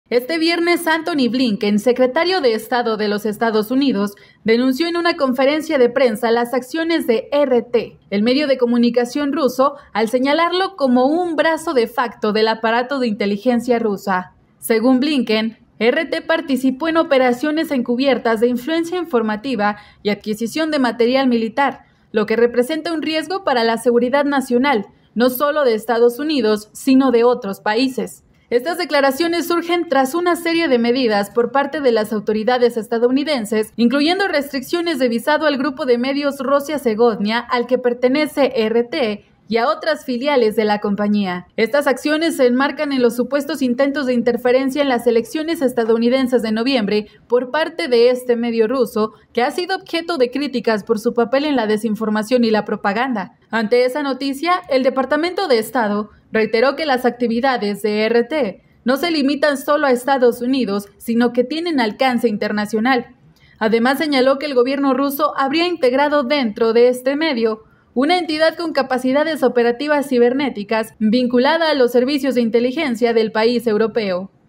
Antony Blinken denuncia al canal ruso RT de intervenir en las elecciones de EU
Este viernes Antony Blinken, secretario de Estado de los Estados Unidos, denunció en una conferencia de prensa las acciones de RT, el medio de comunicación ruso, al señalarlo como un “brazo de facto del aparato de inteligencia de Rusia”.